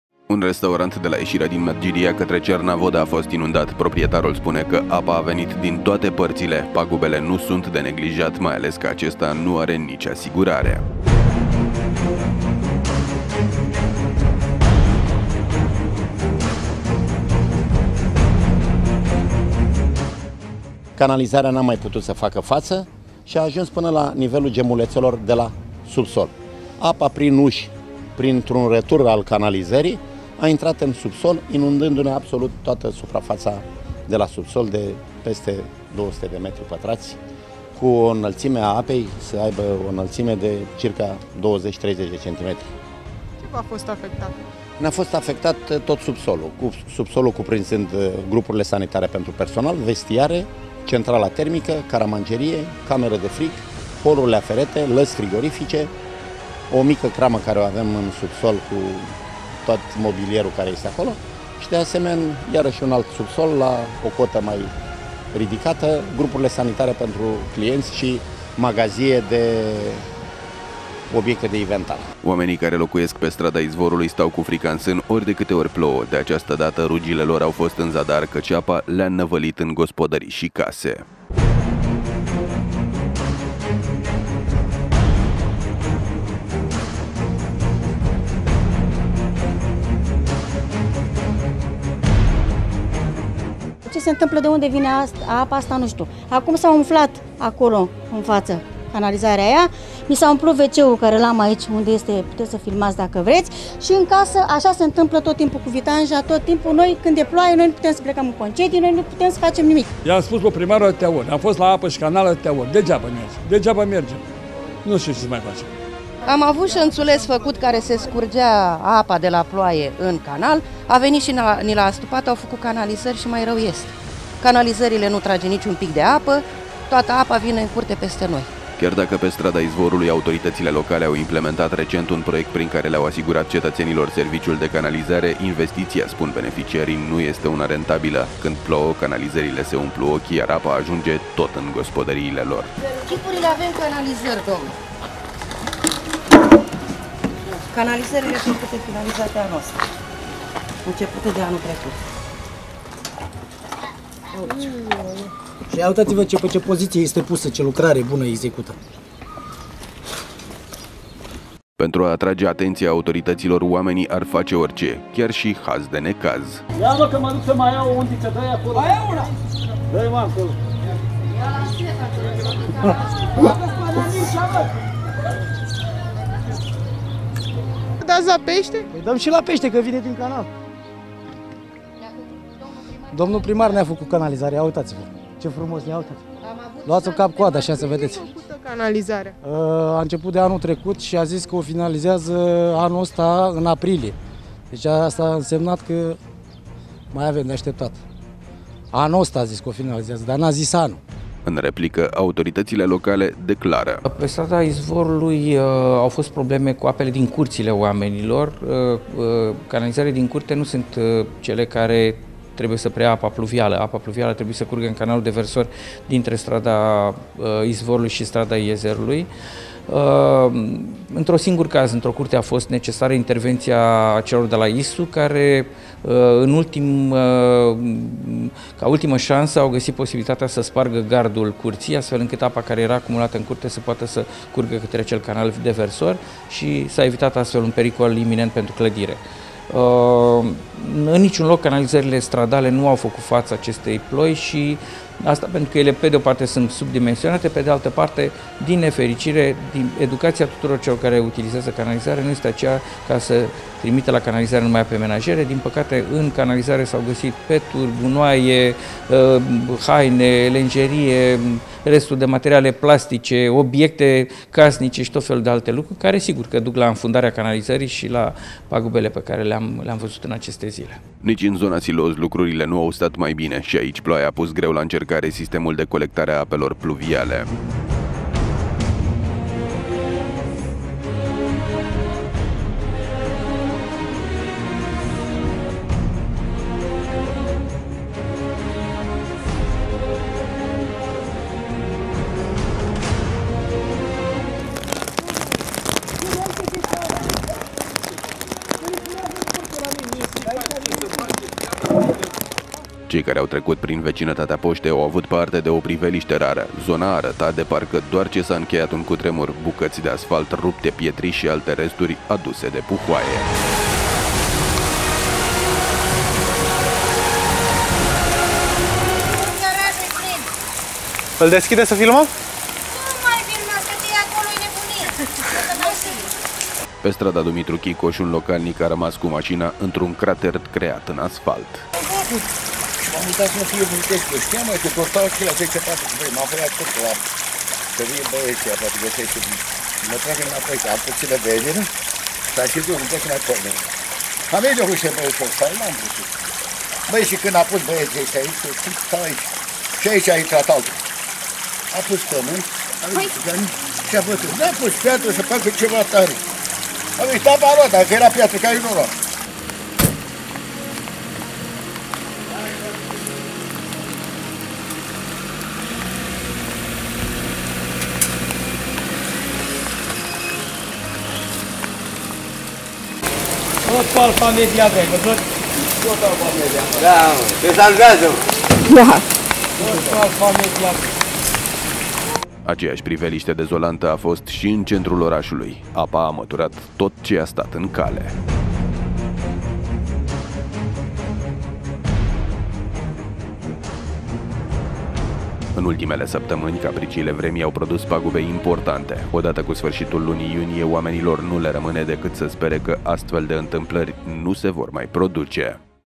A MĂTURAT TOT CE I-A STAT ÎN CALE
Natura nu a iertat pe nimeni! În zona de nord a Medgidiei puhoaiele de apă au lăsat prăpăd în urmă lor! Disperaţi, oamenii încearcă să găsească vinovaţi! Natura dezlănțuită nu a mai ținut cont nici măcar de kilometrul zero al orașului!